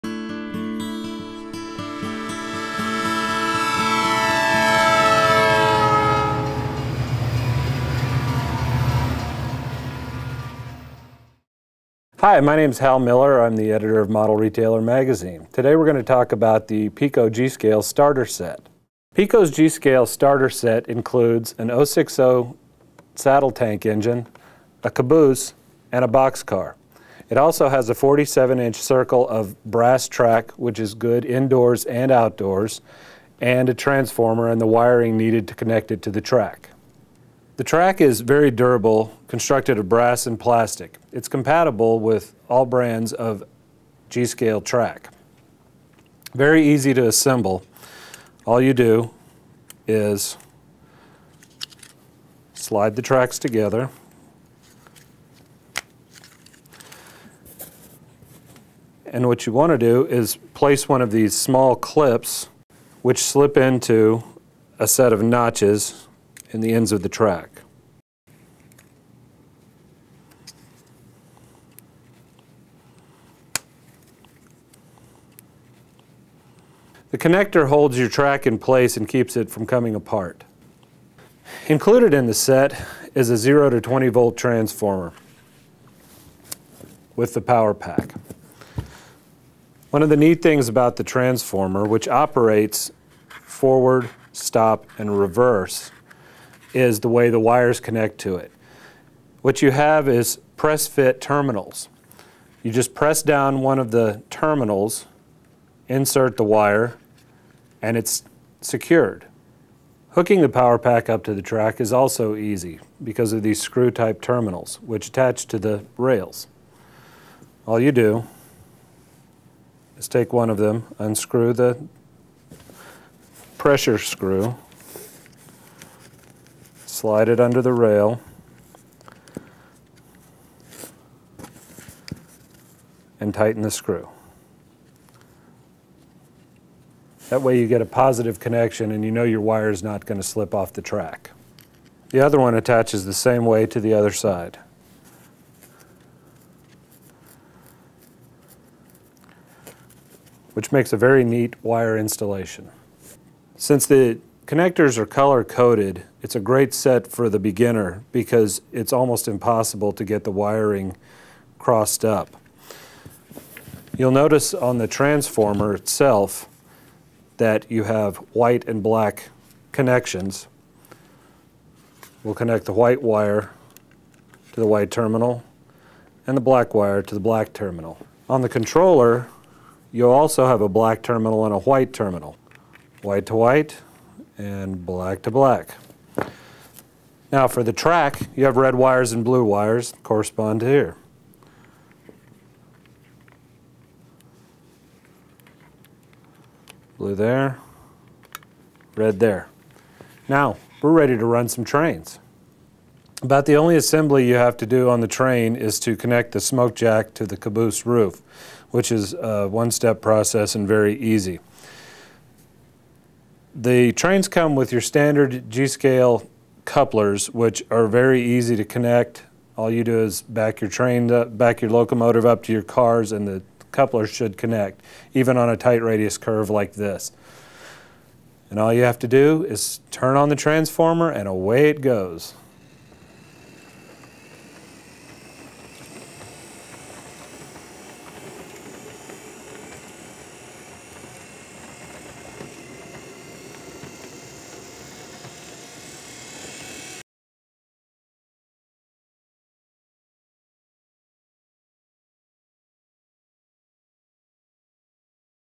Model Railroader magazine Piko G Scale train set review
Model Retailer Editor Hal Miller shows how to set up, wire, and operate this German-made G scale model train set that features an 0-6-0 steam locomotive, two cars, and a circle of track plus a power pack.